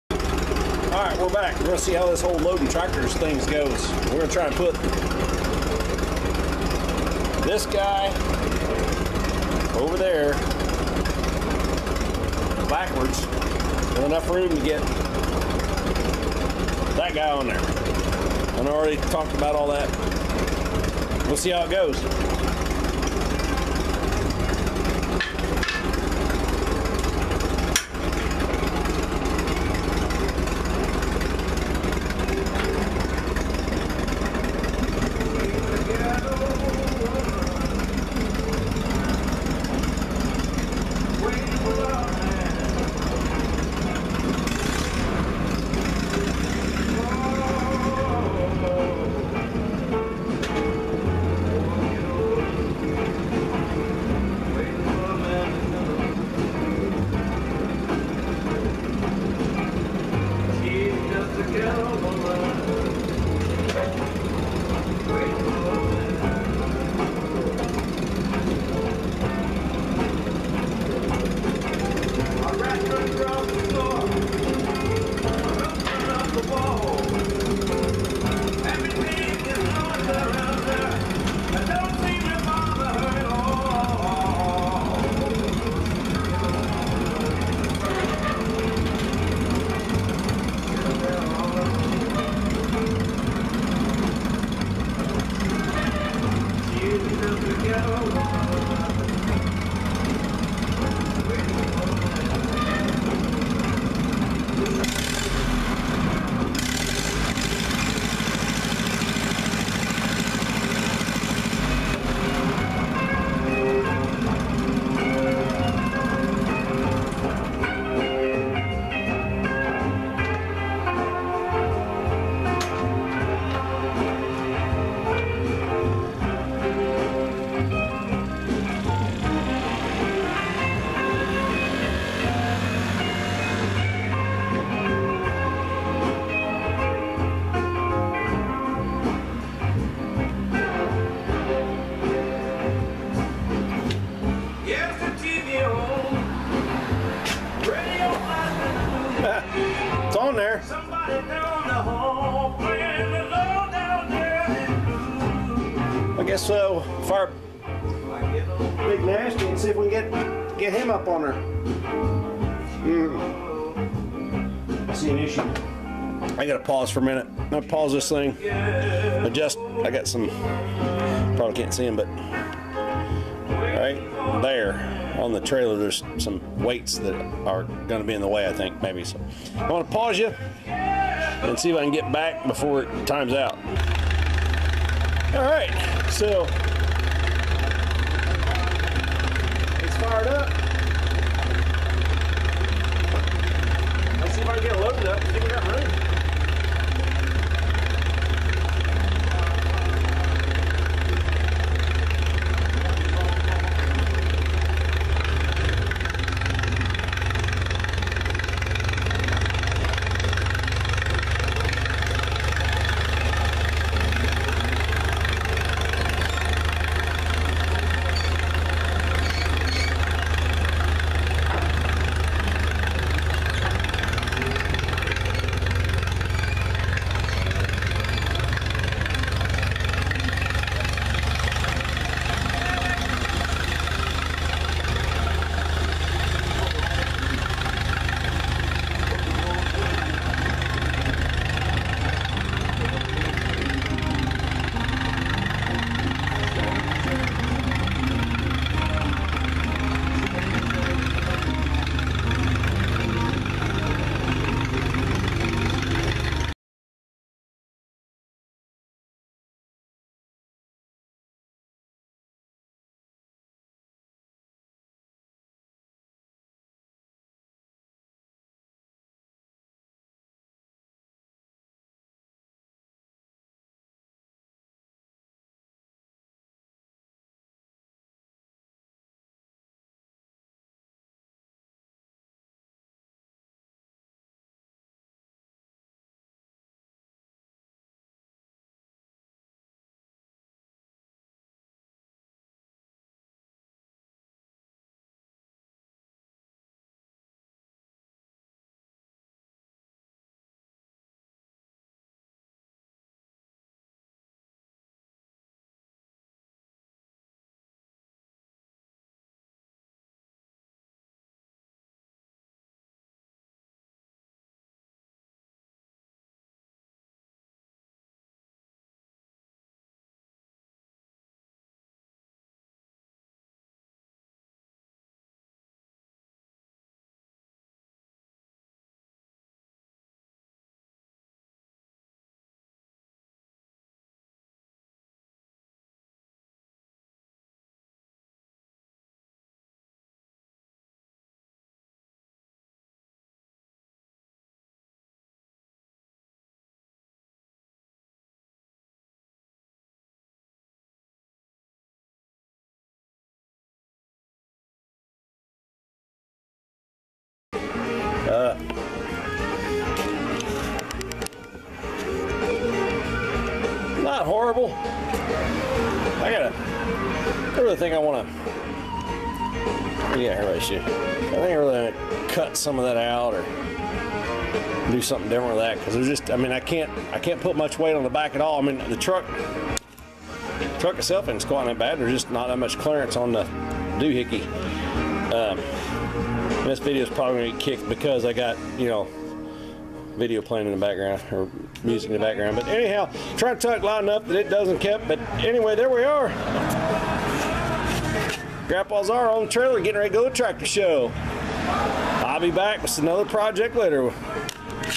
Grandpa's R part 37
out of the garage and on the way to the PGR show in Tipton